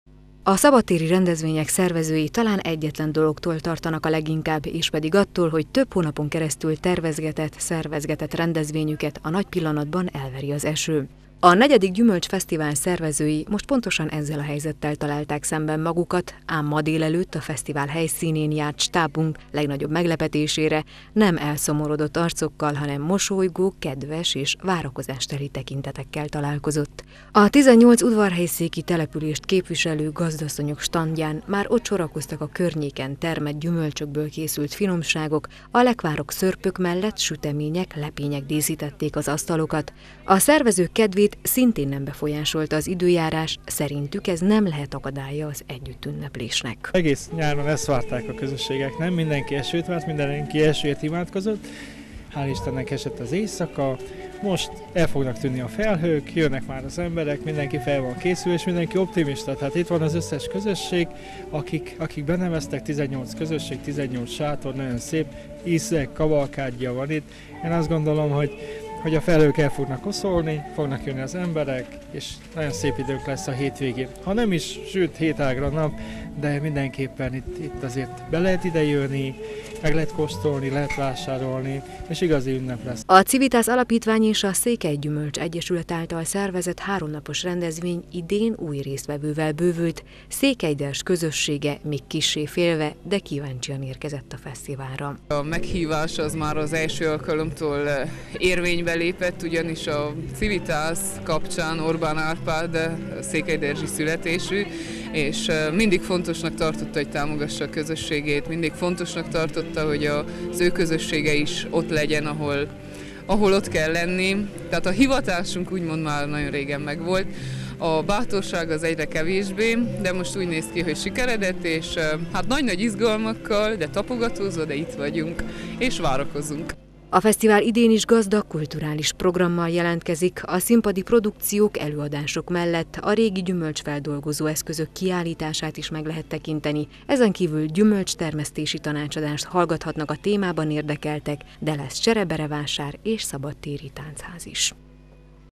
Vár a Gyümölcsfesztivál
Rég várt égi áldással indult ma az ősz egyik legnépszerűbb, hagyományos értékrendet őrző fesztiválja, a negyedjére jelentkező Gyümülcsfesztivál. A Digital3 Televízió stábja délelőtt látogatott ki a rendezvénynek otthont adó városi minifoci pályára, a hűvös, esős időjárás ellenére valamennyi stand finomságokkal megpakolva, készen állt a látogatók fogadására. A háromnapos fesztiválra ingyenes a belépés.